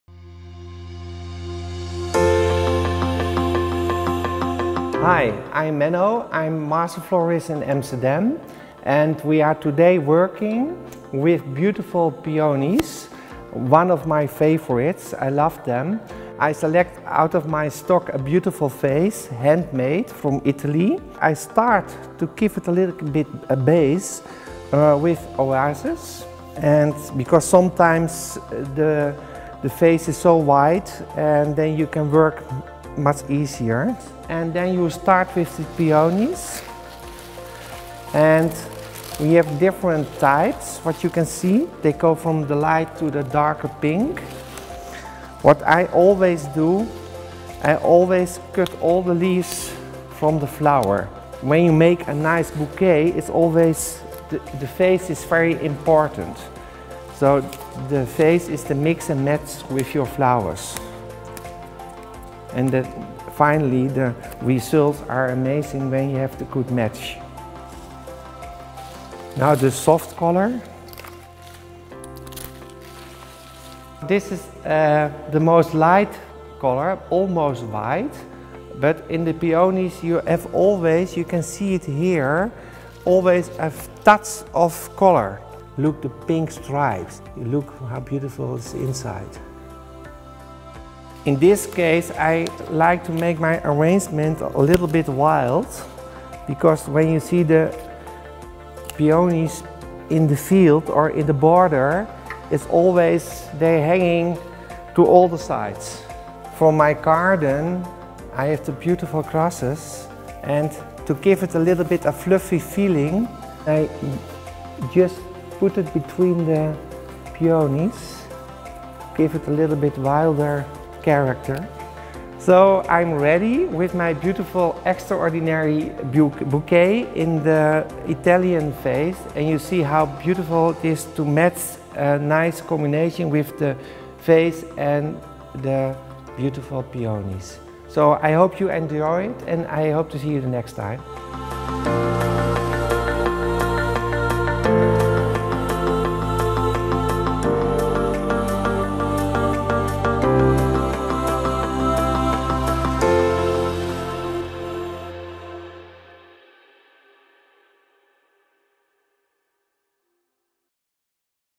Italian-style peonies | Flower Factor tutorial | Powered by My Peony Society
Dutch master florist Menno Kroon shows how he likes to mix and match flowers and vases. For a big bunch of gorgeous peonies he chooses to work with an extraordinary handmade Italian vase and the result is stunning!